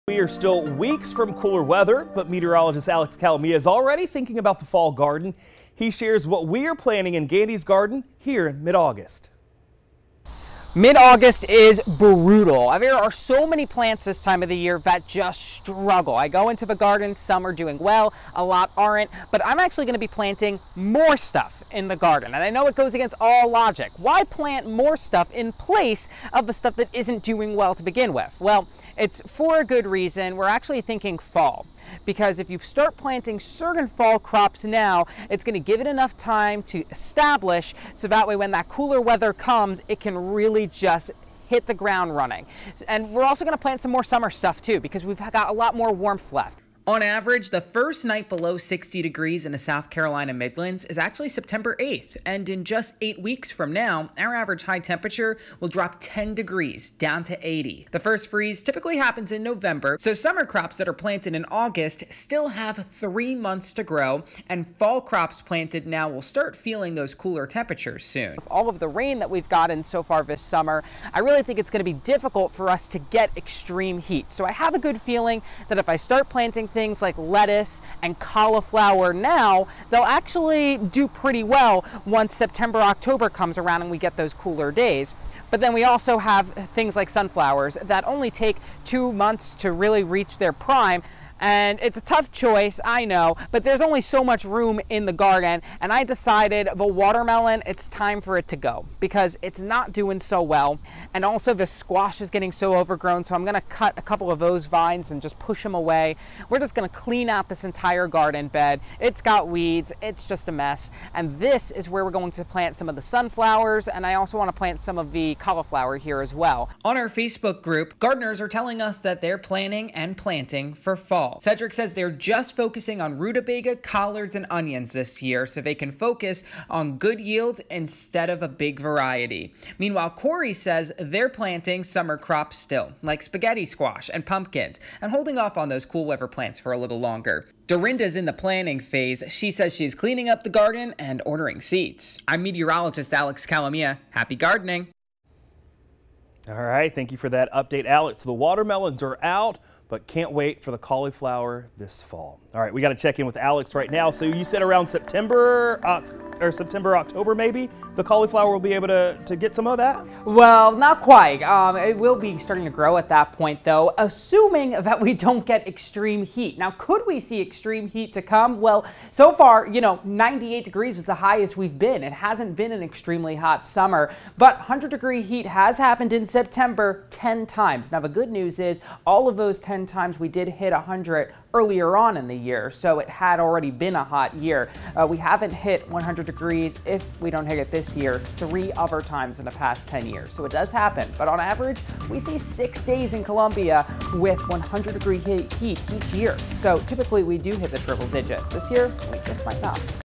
It's Mid-August, but time to think "Fall" in the garden
South Carolina typically won't see a taste of fall weather until October, but summer is the time to start planning. Here's how to end 2020 with a garden.